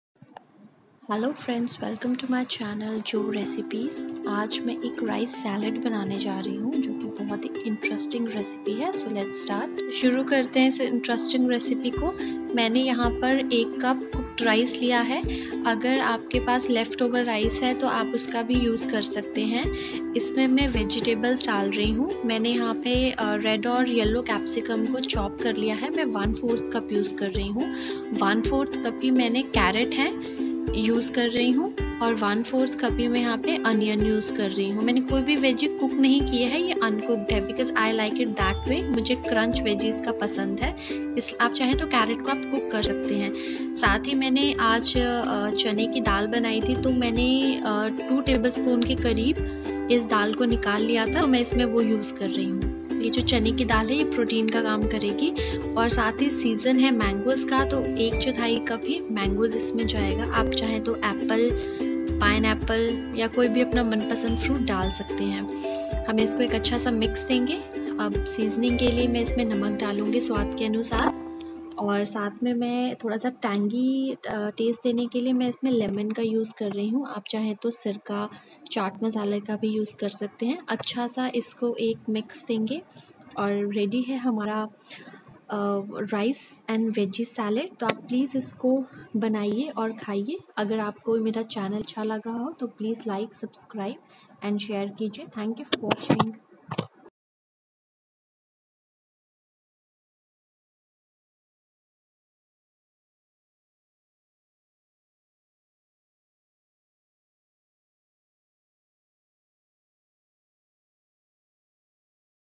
Rice Veggies Salad# salad for weight loss# salad for dinner# veggies for dinner# health for life
This salad helps to manage weight, perfect for dinner, protein, carbs, sweet, salty , tangy. It has all the flavours altogether. Do try this and manage your weight.